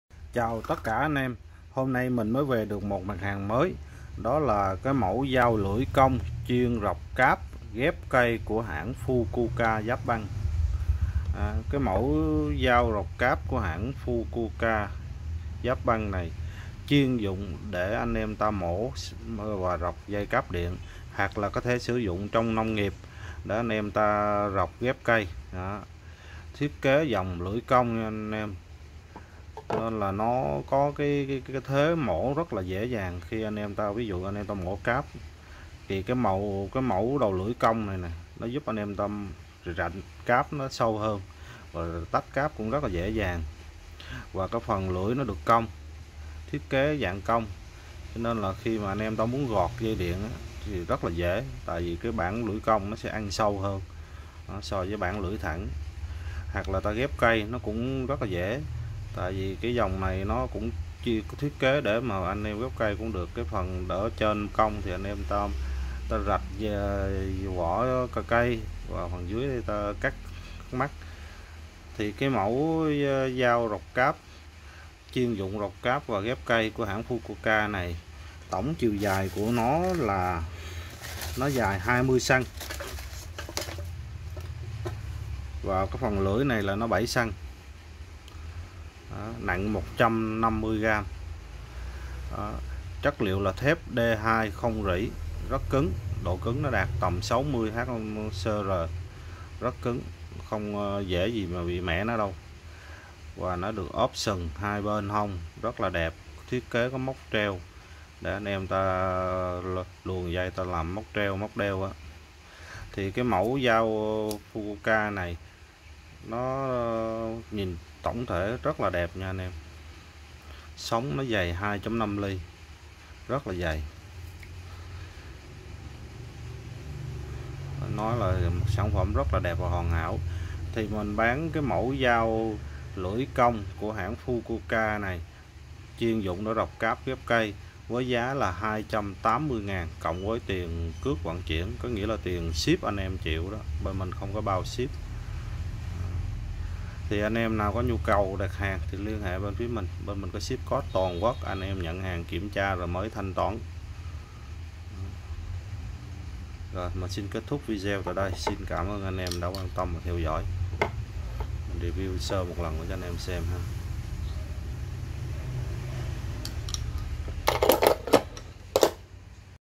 dao rọc cáp ,ghép cây FUKUOKA Nhật Bản giá 280k+phí vận chuyển (lh đại 0966425154)
Giới thiệu anh em mẫu dao chuyên dụng ,dùng để rọc cáp ,ghép cây của hãng FUKUOKA Nhật Bản ,dòng lưỡi cong chuyên dụng mổ cáp ,tổng chiều dài 20cm,lưỡi dài 7cm,nặng 150g.ốp sừng 2 bên,thiết kế có móc treo tiện dụng.rất thích hợp cho anh em làm nghề điện,hoặc ghép cây. Giá 280k+phí vận chuyển /1 cây. Anh em quan tâm lh đại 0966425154-ship cod toàn quốc